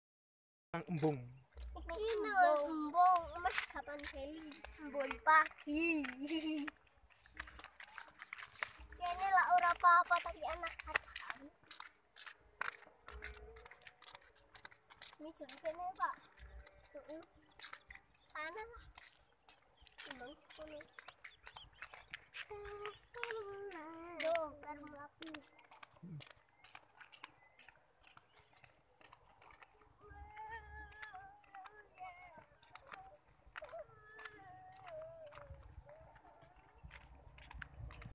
Obyek wisata Embung Kledung,,, temanggung
Embung kledung,,, berada di wilayah kab. Temanggung,,jl parakan,, kledung,, sekitar 3 meniy dari jalan raya